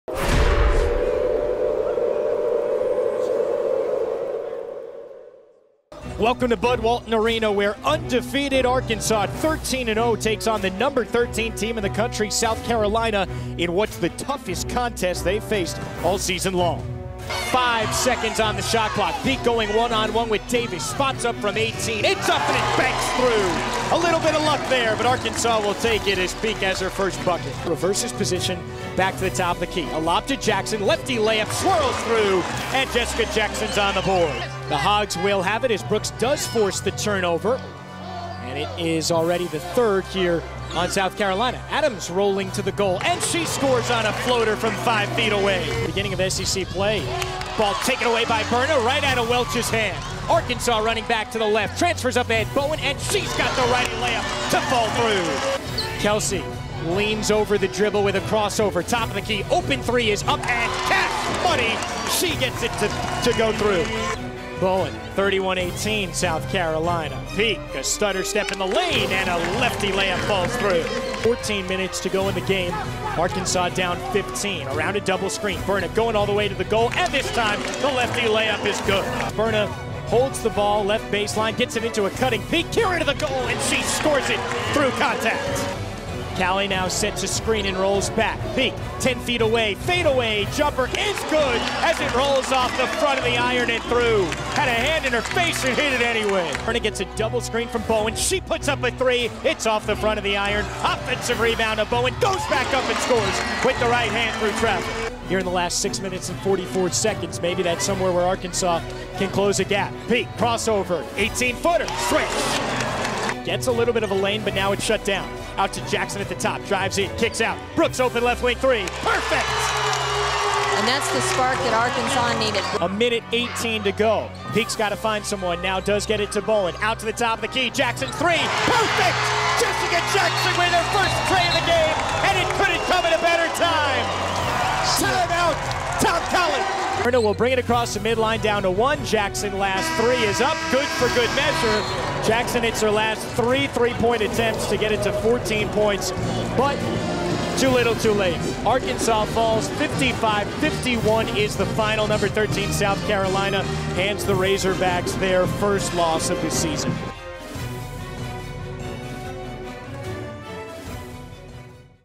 rally falls short vs. #13 South Carolina
Highlights from Arkansas' 55-51 loss to South Carolina